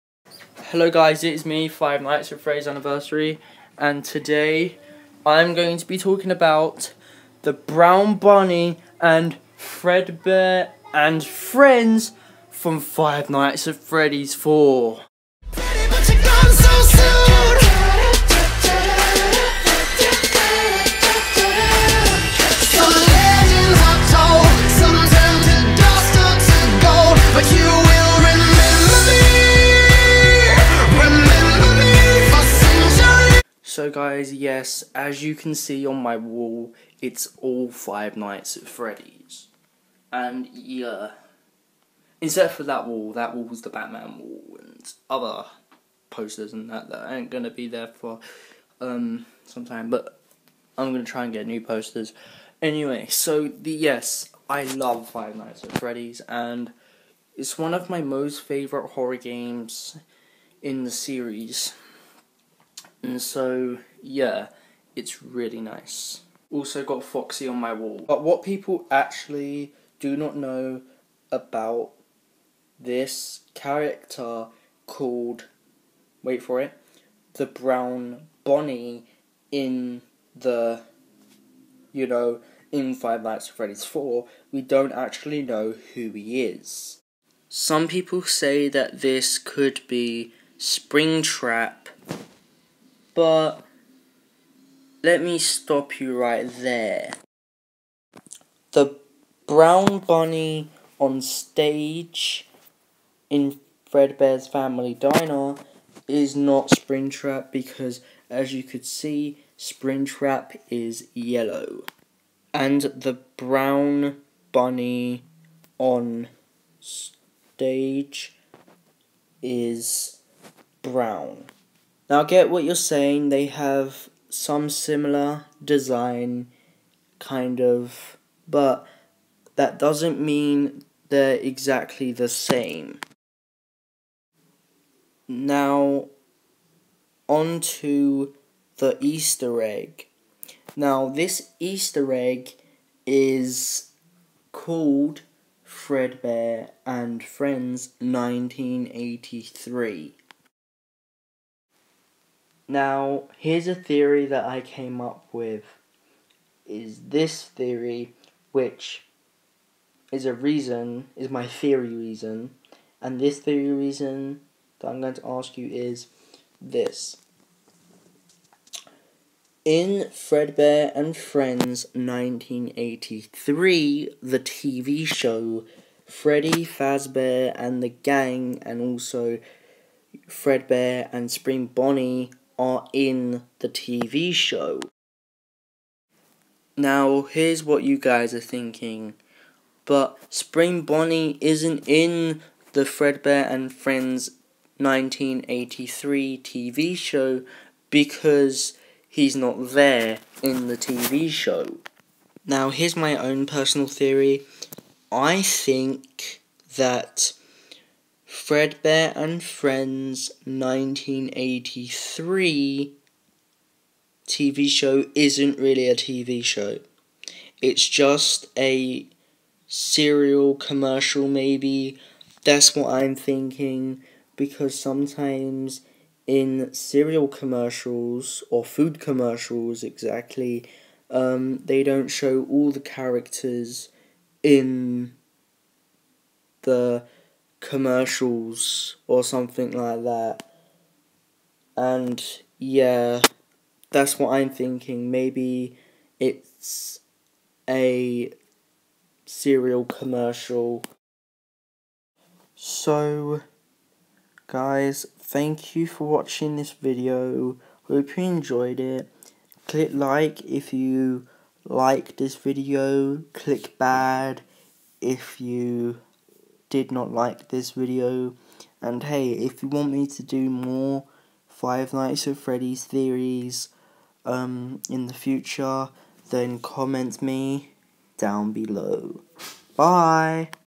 Fnaf 4 Theory Who Is The Brown Bonnie On The Show Stage.
This will fetcher the fredbear and friends 1983 tv show